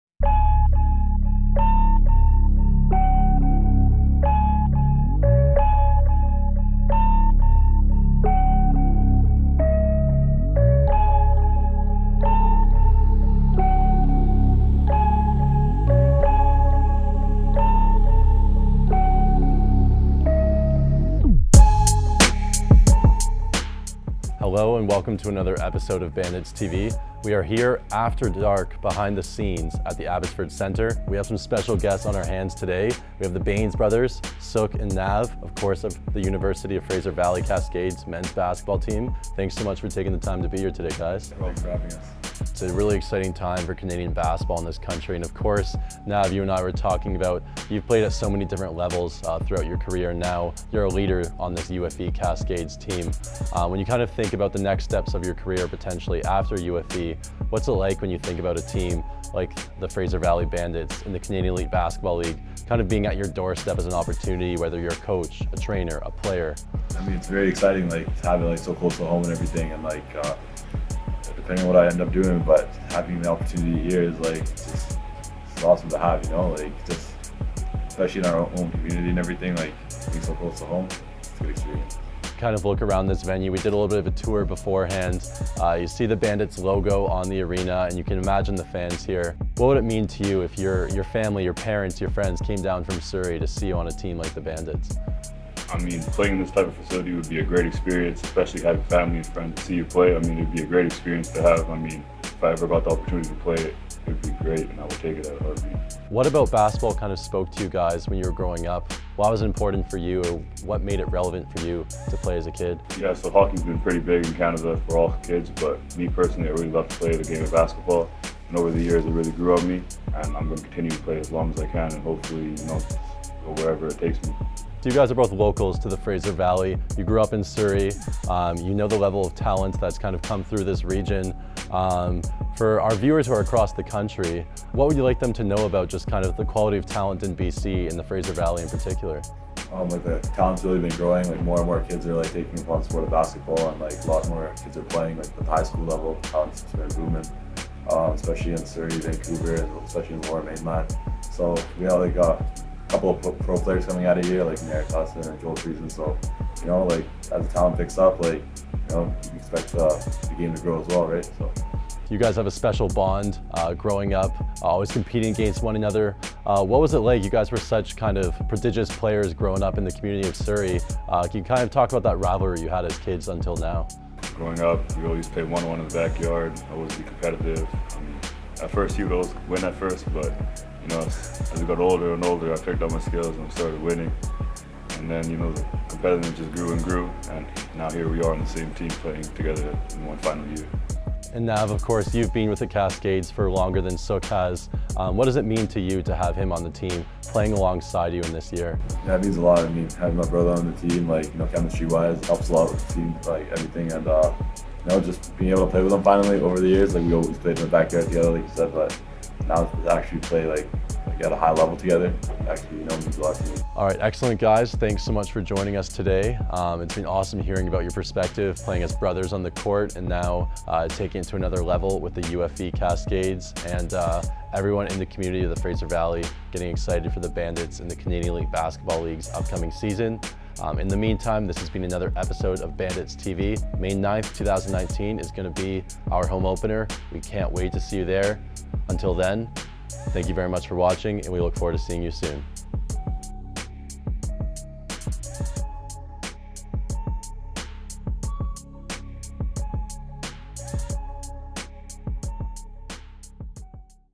Bandits TV S1E2 - Interview with the Bains Brothers
In the second episode of Bandits TV, University of the Fraser Valley Cascades stars Sukhjot and Navjot Bains sit down to discuss the impact of professional basketball's return to BC and their special bond on and off the court.

for more exciting content!